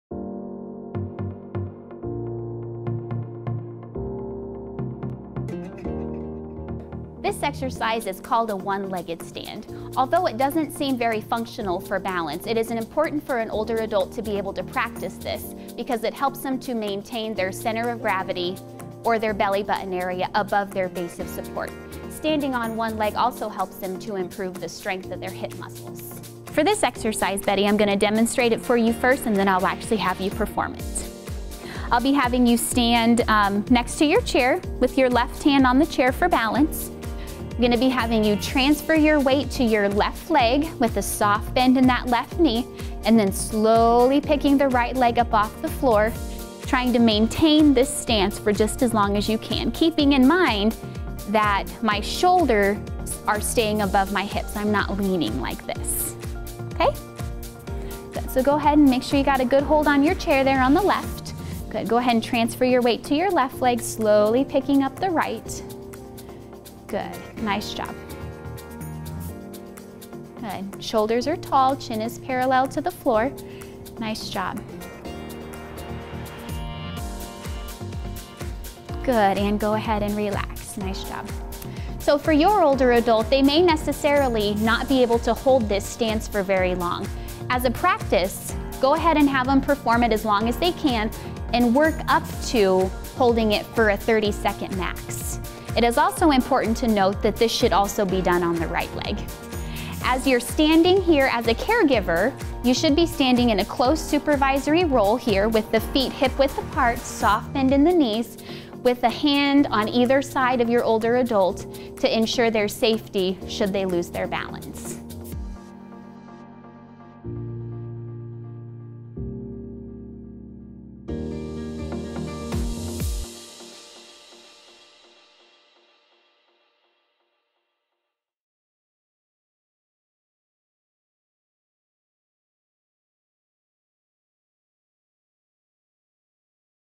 Part 6: One Legged Stand - Prevent Senior Falls: Assessment & Balance Exercises
One legged stands are important for older adults to practice to maintain strength in their center of gravity. Watch the entire "Prevent Senior Falls: Assessment & Balance Exercises" video series on caregiverstress.com